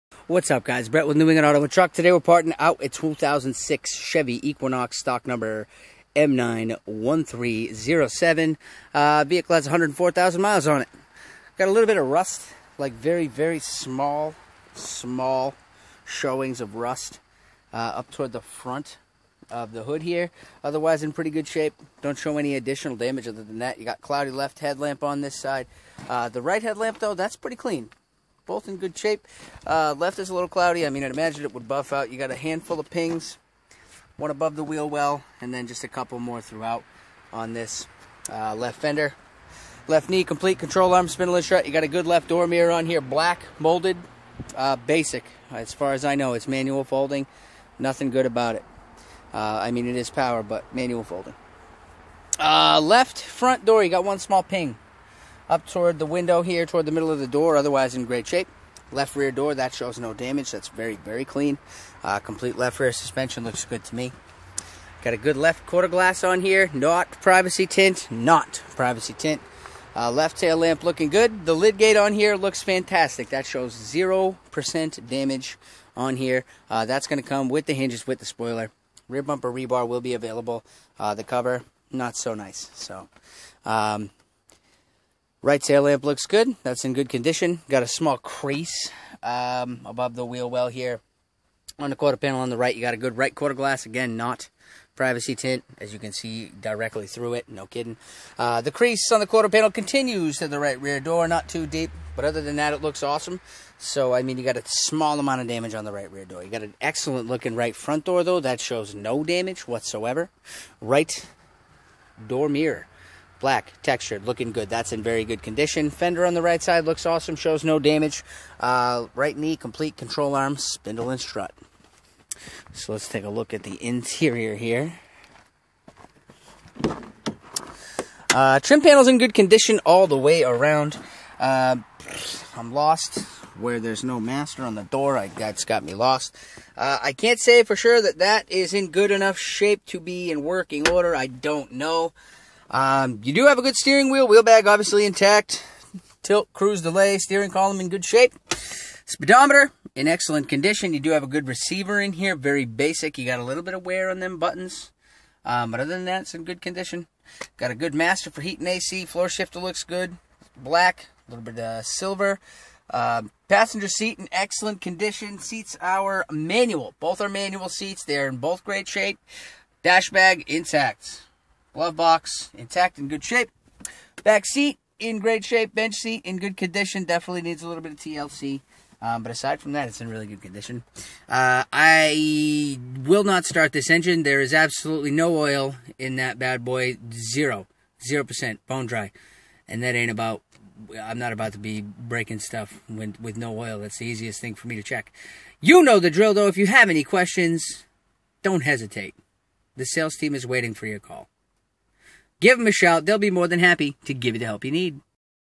PARTING OUT A 2006 CHEVY EQUINOX | STOCK # M91307
NEW ENGLAND AUTO AND TRUCK RECYCLERS IS PARTING OUT A 2006 CHEVY EQUINOX